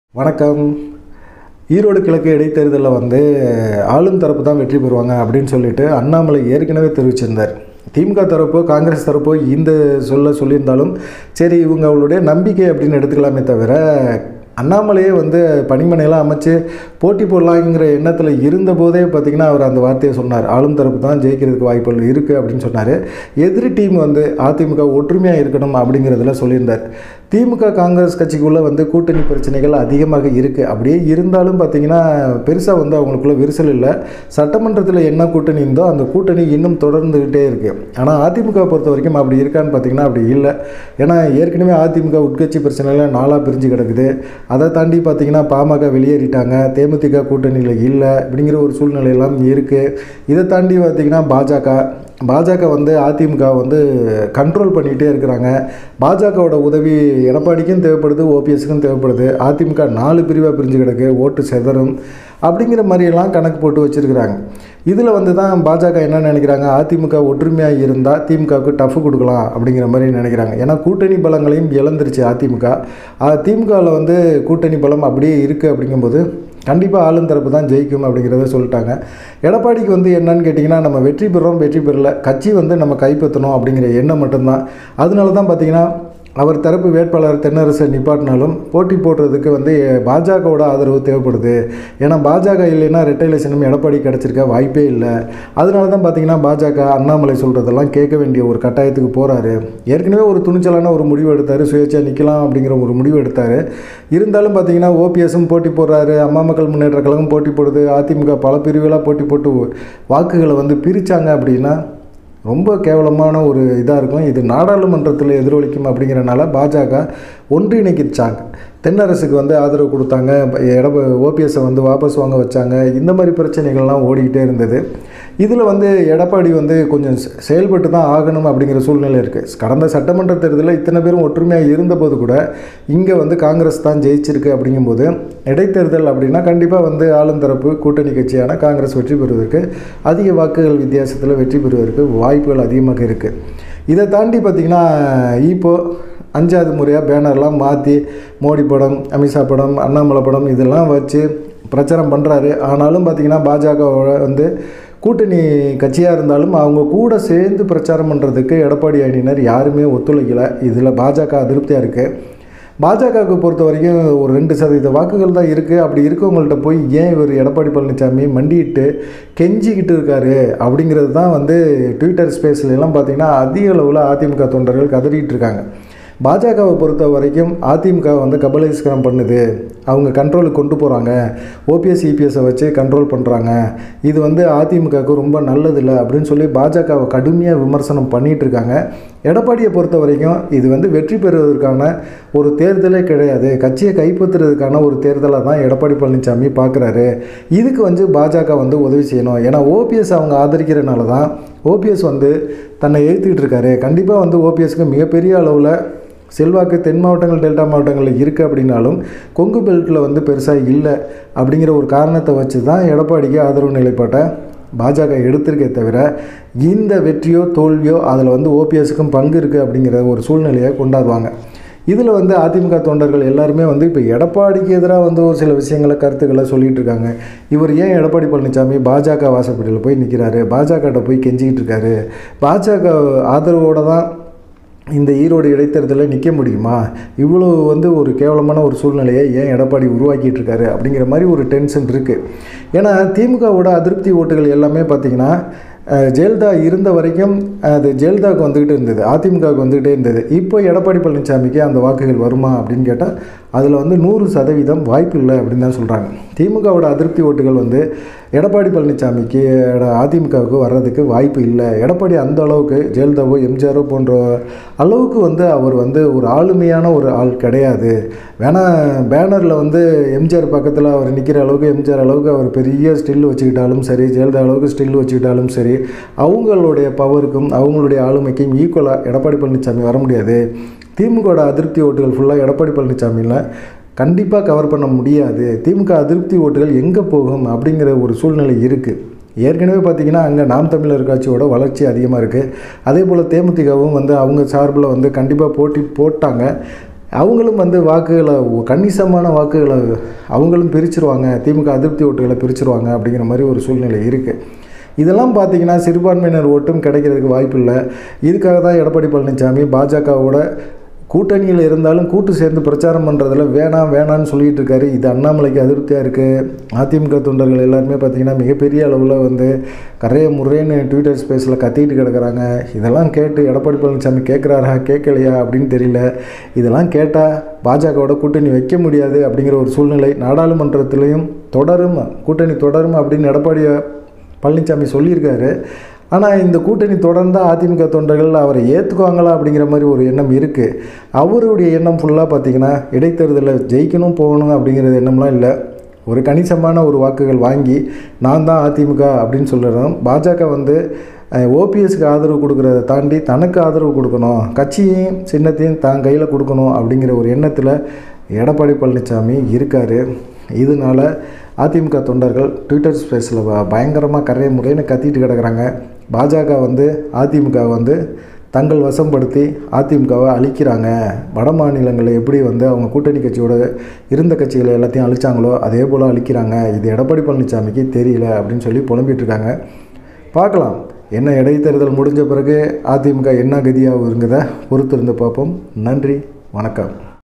திமுக கூட்டணி தான் ஜெயிக்கும் அண்ணாமலை இப்படி சொல்லிட்டாரே?
ஒபிஎஸ், இபிஎஸ்க்கு கட்சி கிடையாதாம்.. பாஜக பிளான் வேறயாம்.. அட கொடுமையே..!